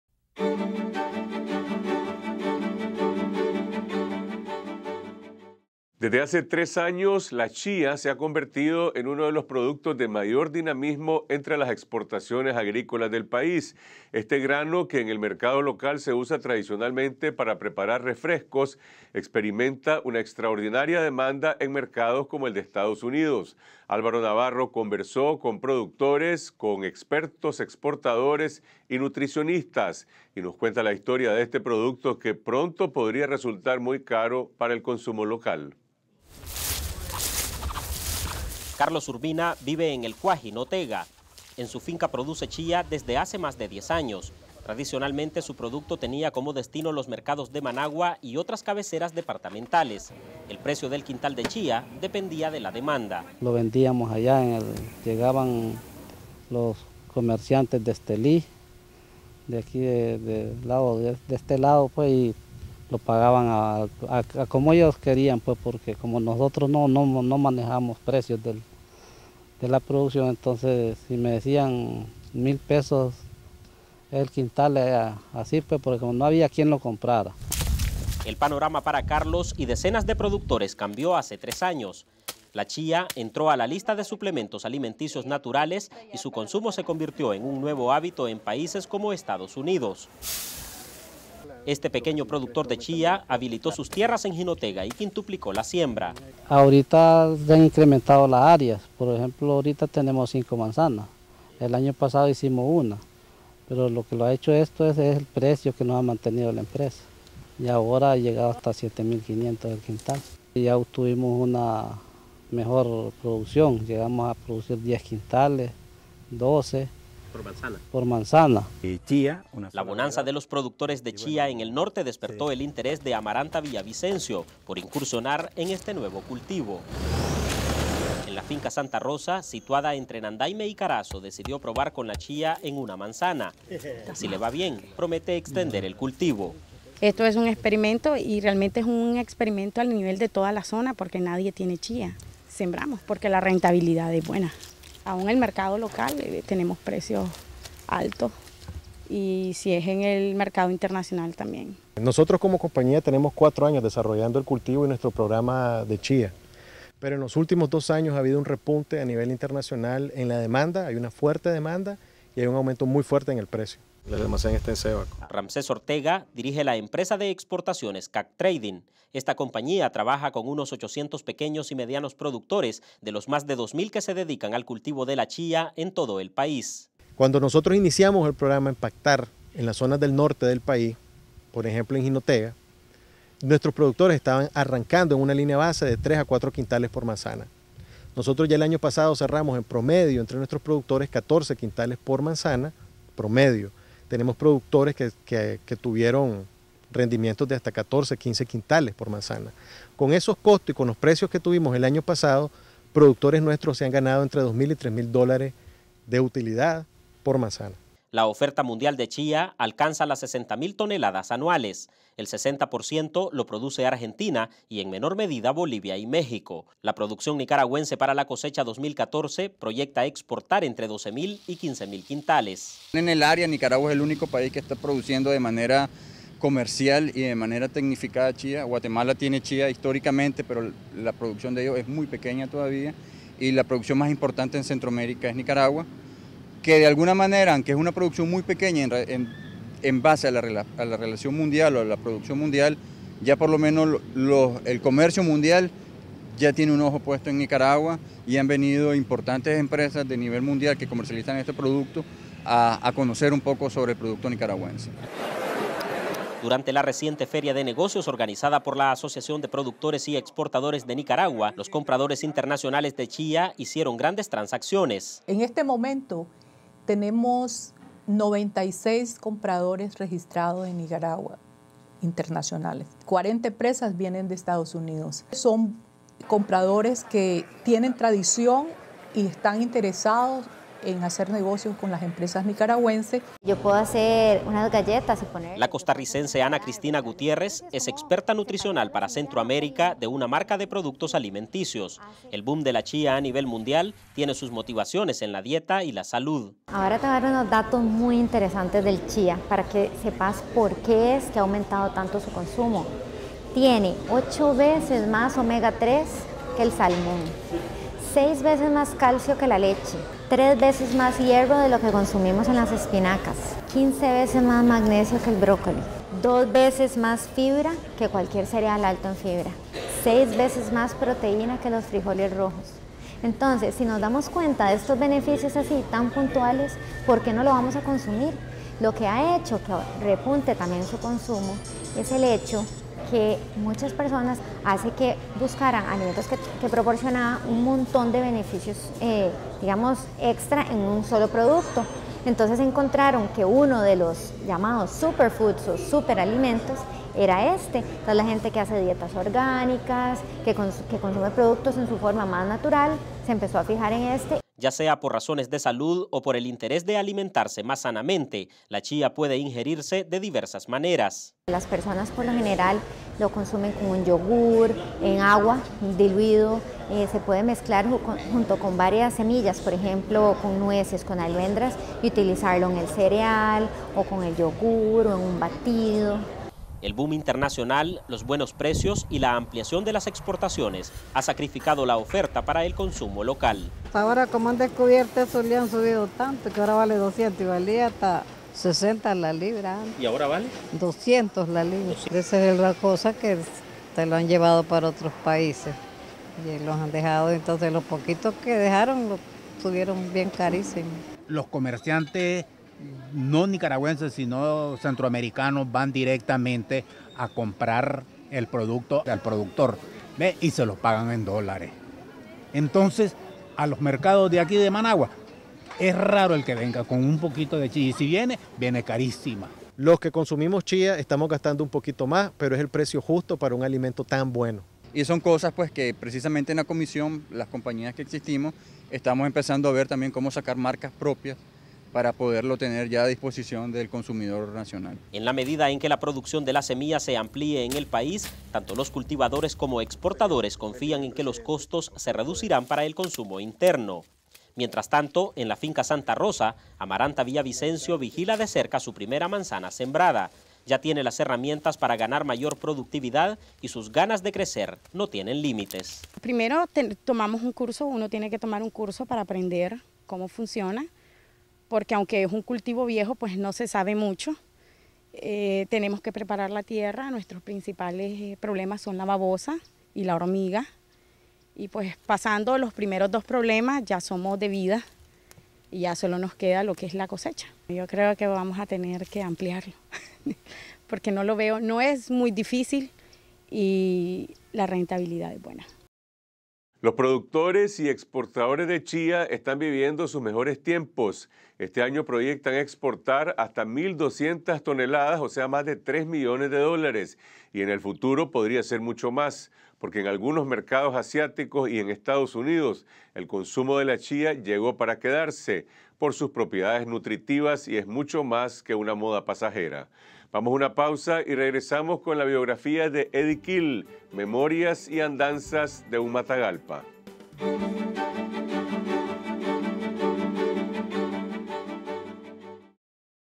Chía: oro en polvo
La chía es la estrella del momento en el firmamento de los productos no tradicionales. Centenares de productores de Jinotega, Matagalpa y Carazo aprovechan la alta demanda de esta nutritiva semilla.

• En tres años, el quintal sube de 45 a 250 dólares, exportaciones crecen de 200 a 1,200 toneladas por año.